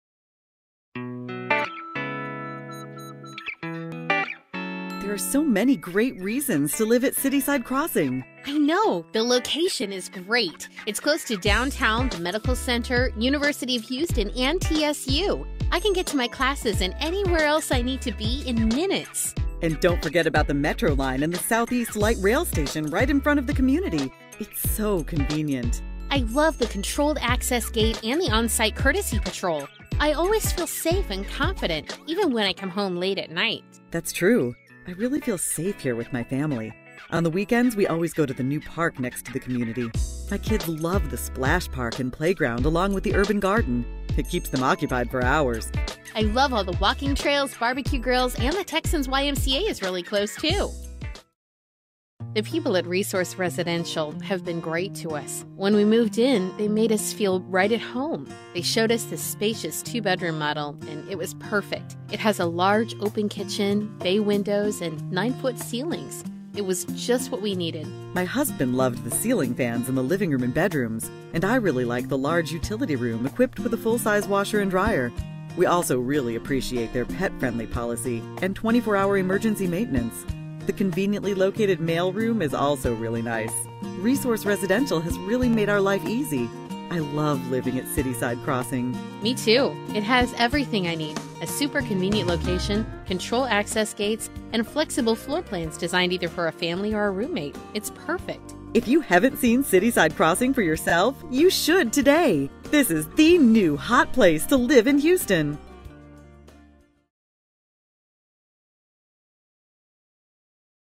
Cityside Crossing Apartment Homes Houston
Come experience Cityside Crossing Apartments, a quiet rental community developed and managed to make your daily living a pleasure.  When you arrive here at Cityside Crossing you'll know this is home!
Residents can enjoy the benefits of living next door to the City Park at Palm Center.  The park features a splash pad, playground, outdoor kitchen, urban garden, farmer's market, and walking trails.  Cityside Crossing is within walking distance from the Houston Texans YMCA.  The YMCA features study area with wifi, outdoor pool, splash pad, sport leagues, fitness classes, basketball court, fitness center, teen computer room, planned activities, child care and much more.
Coming Soon: Metro Light Rail SE Line will open in 2014.  The transit station is located at the community entry.  The rail system will take residents to downtown, TSU, U of H, Medical Center, and anywhere else the Metro lines travel!